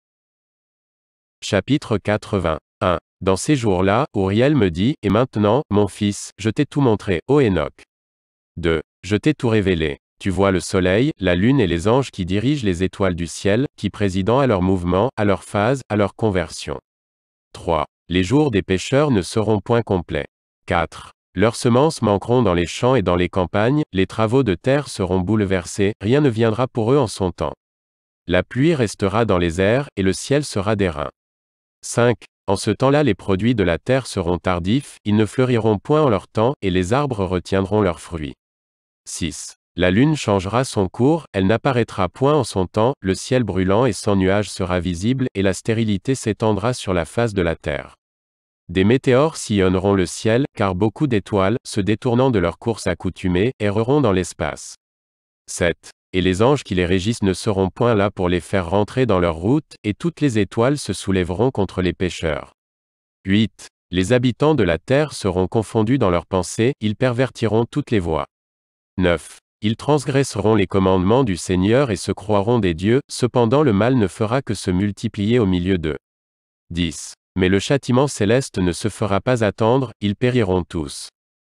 La Bible Audio en Français : Le Livre d'Énoch - Chapitre 80
1. Dans ces jours-là, Ouriel me dit: Et maintenant, mon fils, je t'ai tout montré, ô Enoch.
2. Je t'ai tout révélé. Tu vois le soleil, la lune et les anges qui dirigent les étoiles du ciel, qui président à leurs mouvements, à leurs phases, à leurs conversions.
3. Les jours des pécheurs ne seront point complets.
4. Leurs semences manqueront dans les champs et dans les campagnes; les travaux de terre seront bouleversés, rien ne viendra pour eux en son temps. La pluie restera dans les airs, et le ciel sera d'airain.
5. En ce temps-là les produits de la terre seront tardifs; ils ne fleuriront point en leur temps, et les arbres retiendront leurs fruits.
6. La lune changera son cours, elle n'apparaîtra point en son temps; le ciel brûlant et sans nuages sera visible, et la stérilité s'étendra sur la face de la terre. Des météores sillonneront le ciel; car beaucoup d'étoiles, se détournant de leur course accoutumée, erreront dans l'espace.
7. Et les anges qui les régissent ne seront point là pour les faire rentrer dans leur route; et toutes les étoiles se soulèveront contre les pécheurs.
8. Les habitants de la terre seront confondus dans leurs pensées; ils pervertiront toutes les voies.
9. Ils transgresseront les commandements du Seigneur et se croiront des dieux; cependant le mal ne fera que se multiplier au milieu d'eux.
10. Mais le châtiment céleste ne se fera pas attendre: ils périront tous.